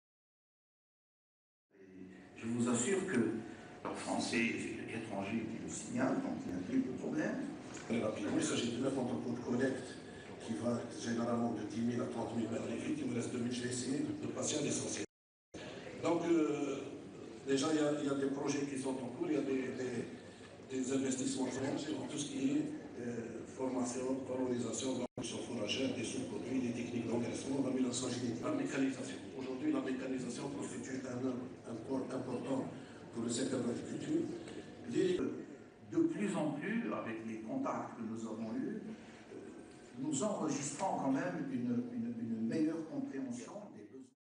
French FM Fabius seen dozing at Algiers conference
Images of French Foreign Minister Laurent Fabius nodding off while attending a conference in Algiers have caused a buzz on social networks.